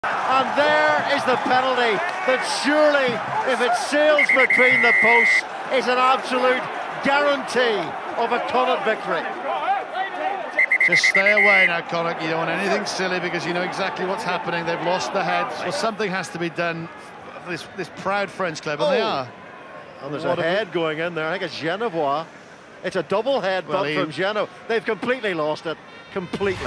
Jean-Philippe Genevois headbutts Ronan Loughney
Biarritz hooker Jean-Philippe Genevois headbutts Connacht prop Ronan Loughney as he gets frustrated as his side concede a penalty on their way to slumping to a 22-14 defeat to the Galway based province in their Heineken Cup match in December 2012.

Astonishingly, despite the evidence, Genevois wasn't cited for the incident.